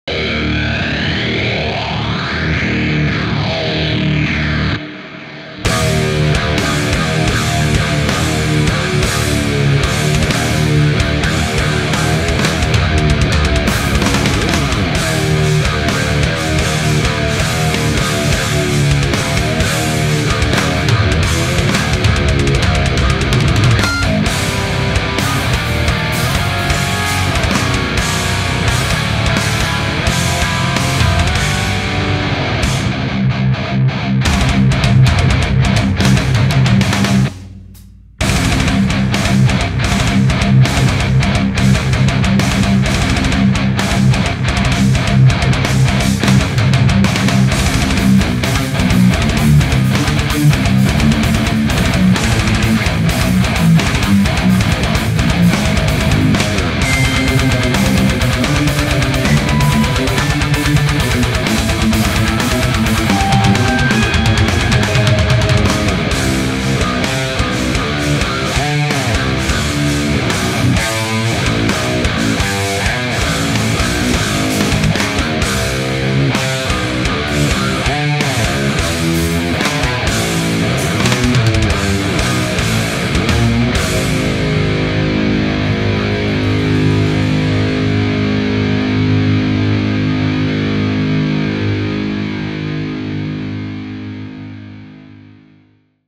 Greg Kubacki (Car Bomb) - Archetype: Gojira demo
Greg Kubacki of Car Bomb demonstrates some monstrous tones using his artist presets in Archetype: Gojira!

Car Bomb are awesome.